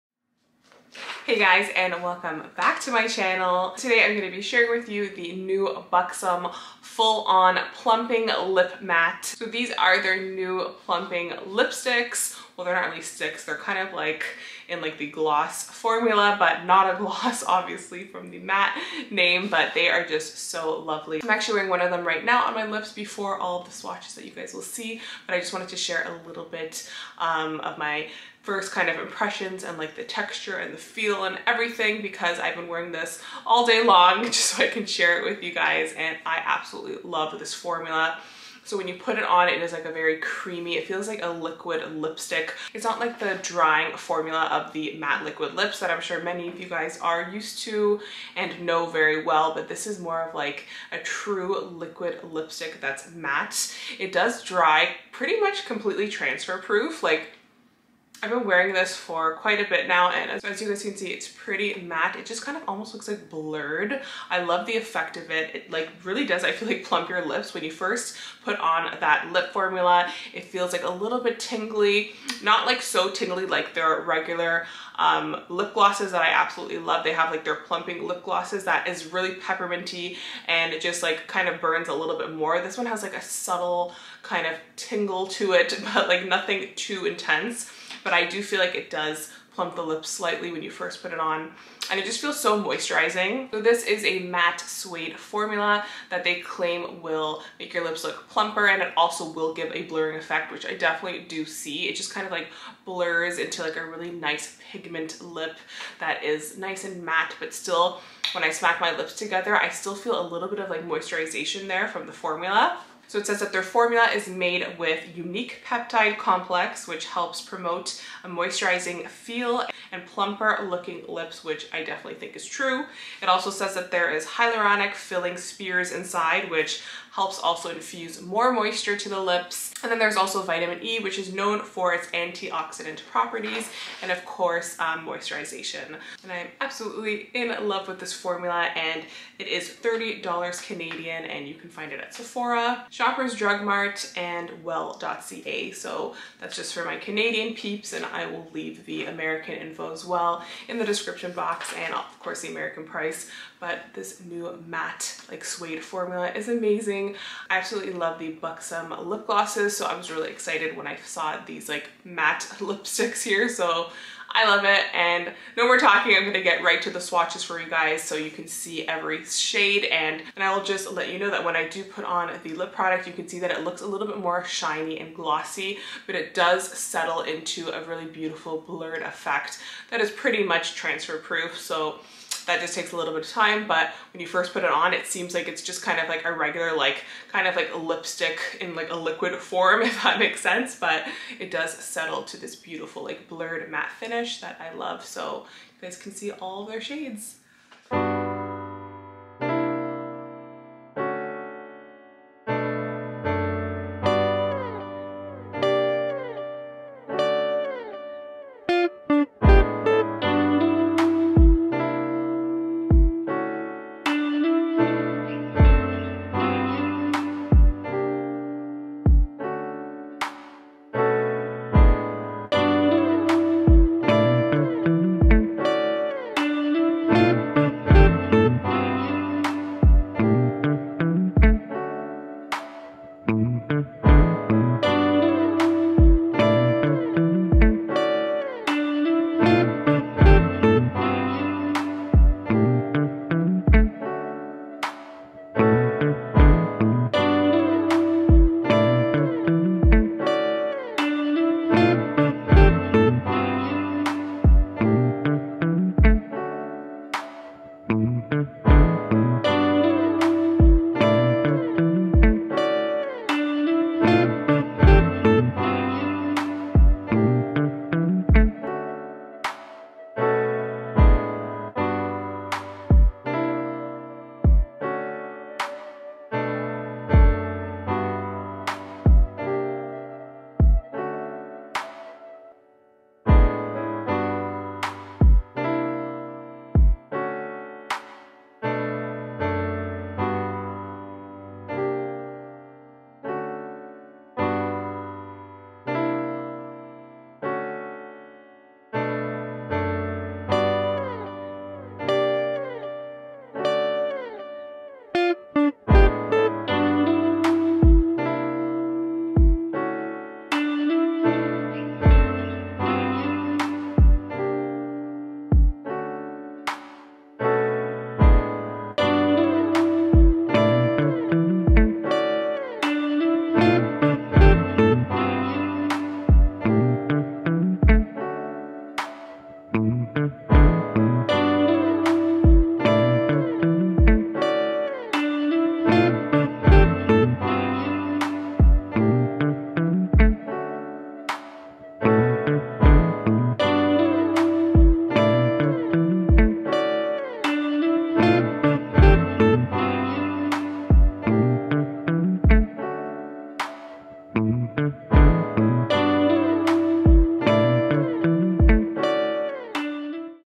NEW! BUXOM "Full-on Plumping Lip MATTE" | Lip Swatches and First Impressions!
I hope you liked this NEW! BUXOM "Full-on Plumping Lip MATTE" | Lip Swatches and First Impressions!

Thank you once again for the support.
Love you guys,
Julia

PRODUCTS USED:
BUXOM "Full-on Plumping Lip MATTE"

FTC: *This video is not sponsored. Products were gifted by Colourpop Cosmetics.